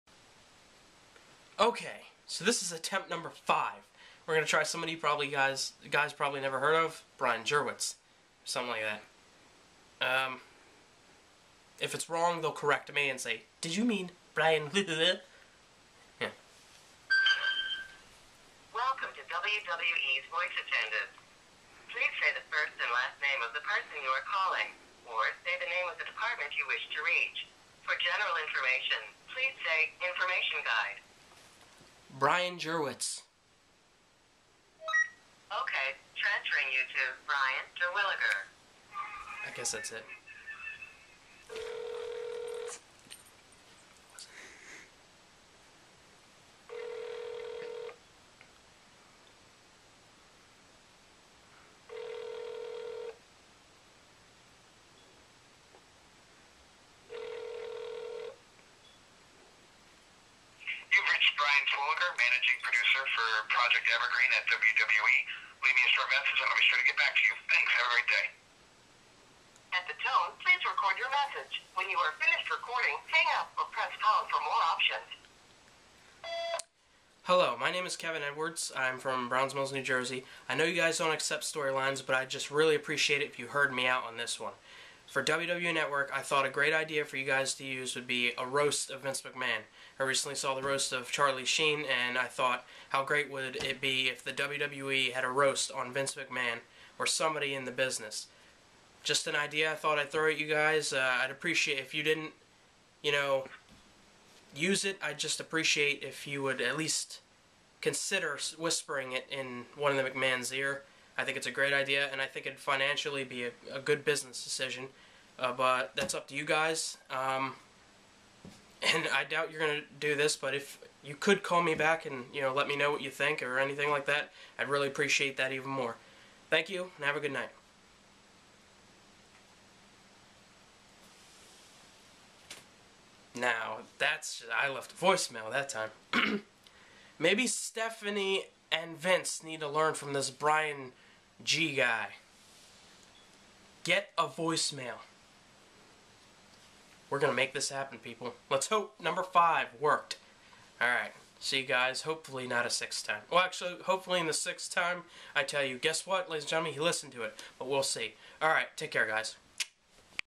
The Vince McMahon Roast: Attempt #5
this is my Fifth attempt to call the wwe Headquarters to either talk to someone or to talk to a voice mail about a possible Vince McMahon Roast when they have the WWE Network Launched... HEY if you know anyone who works for wwe or knows Vince Himself let him know about this idea... lets get this to happen